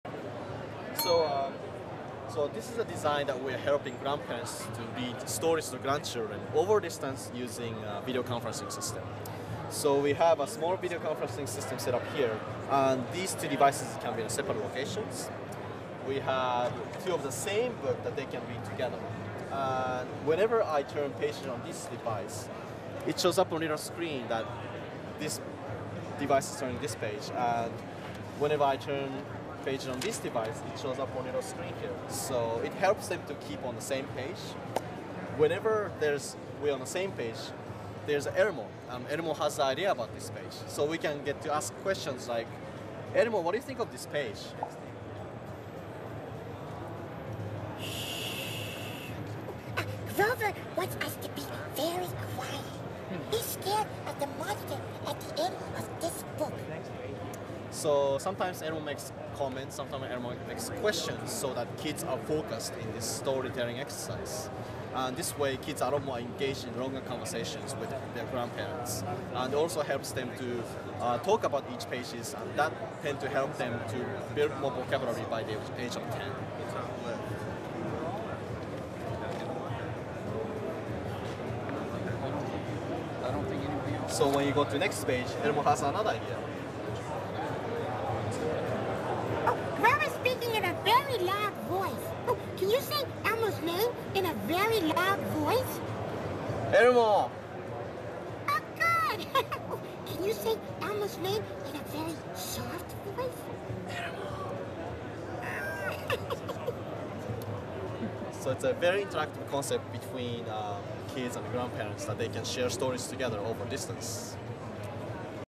Nokia Family Story Play Prototype
Research collaboration between Nokia Research Center and Sesame Workshop has led to this Family Story Play Prototype shown at CES 2010 in Las Vegas!